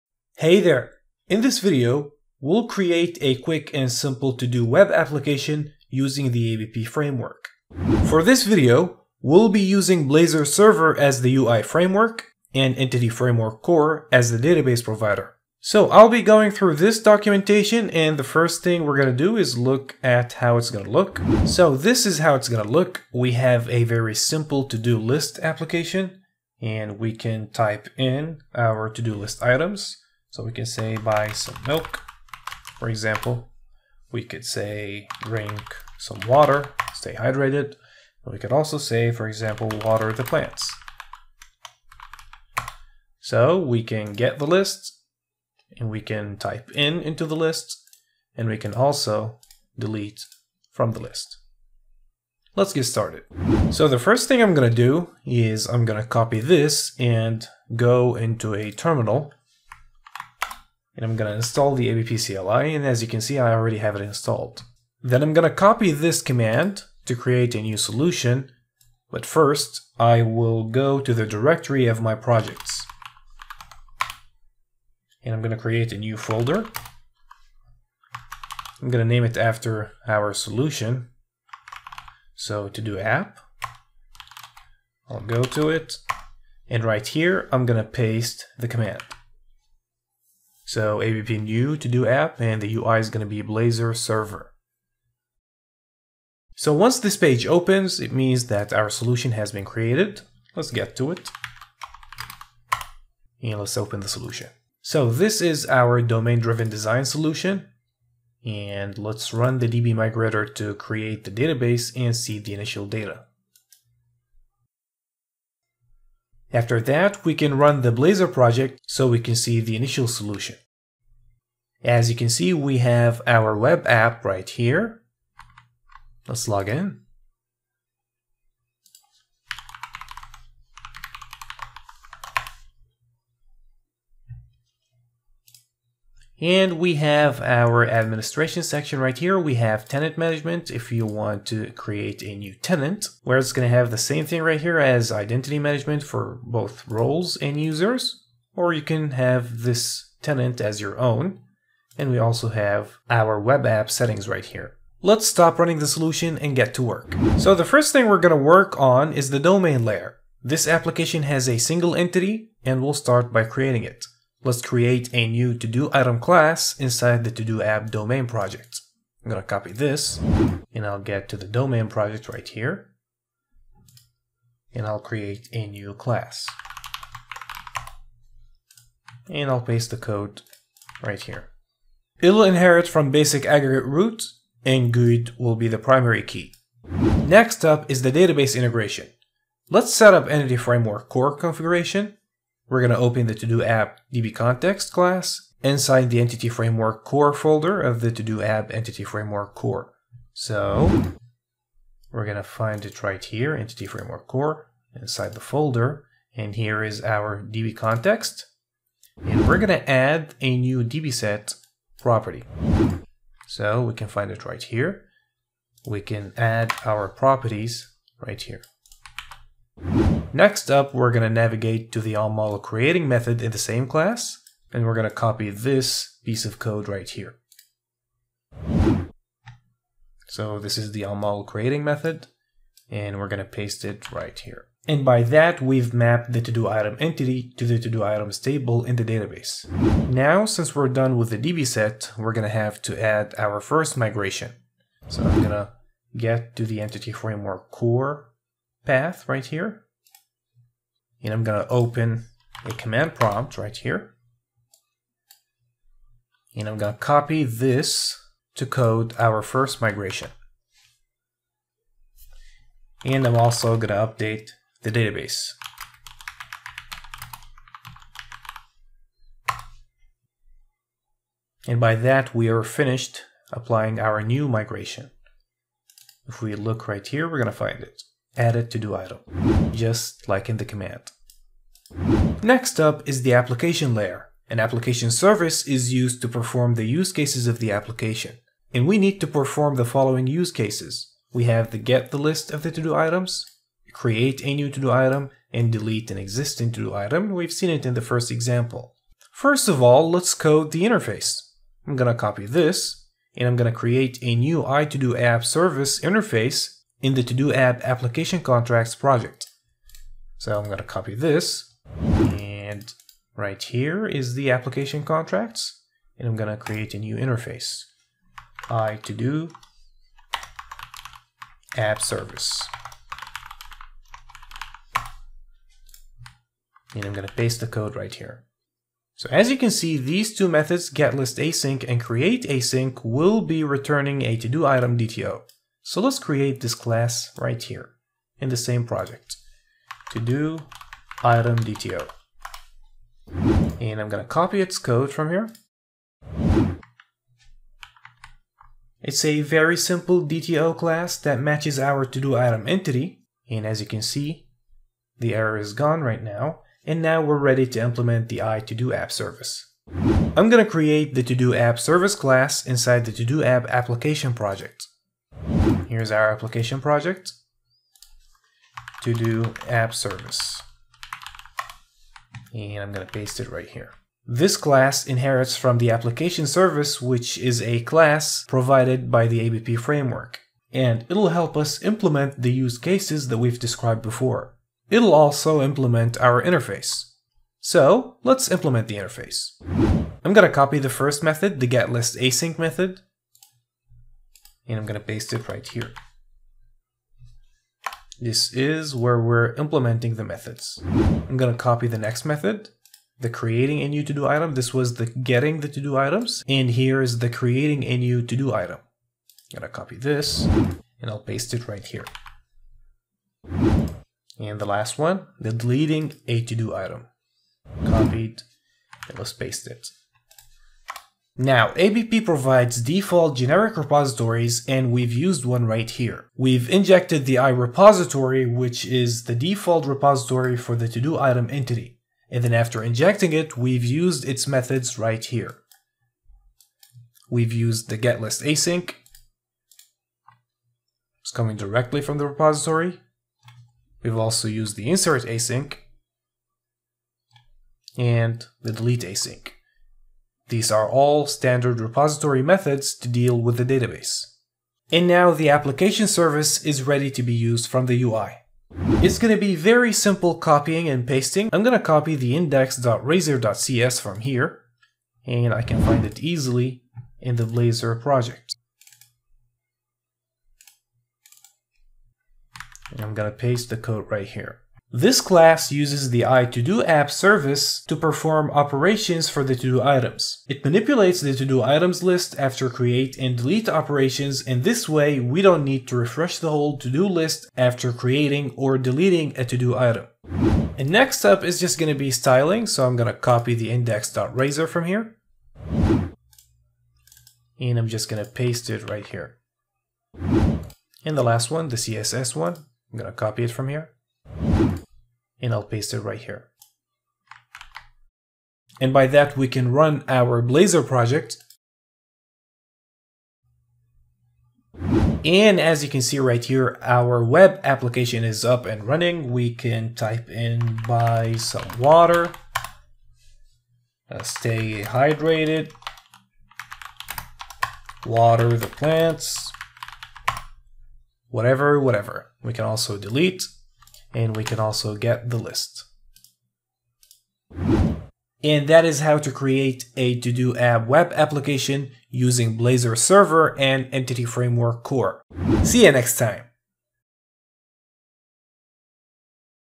Build a Simple To Do App with ABP Framework [Blazor Server + Entity Framework Core]
This is a quick start tutorial to build a simple To Do application with the ABP Framework. We'll use Blazor Server with Entity Framework Core.